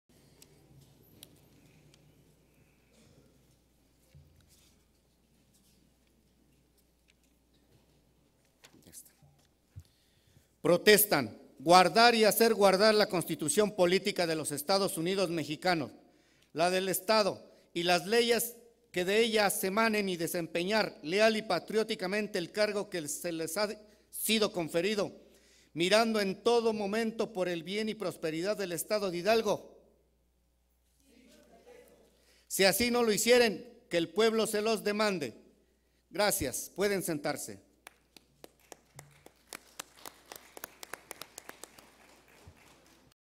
Rinden Protesta las y los integrantes de la Directiva de Diciembre .
Se eligió, con 19 votos a favor, la propuesta de la diputada morenista Doralicia Martínez Bautista relativa a la integración de la directiva del mes de diciembre.
• Presidente: Rafael Garnica Alonso
• Vicepresidente: Areli Rubí Miranda Ayala
• Secretaria Propietaria: Susana Araceli Ángeles Quezada
• Secretaria Propietaria: Tatiana Tonantzín P. Ángeles Moreno
• Suplente General: Julio Manuel Valera Piedras
• Suplente General: Víctor Osmind Guerrero Trejo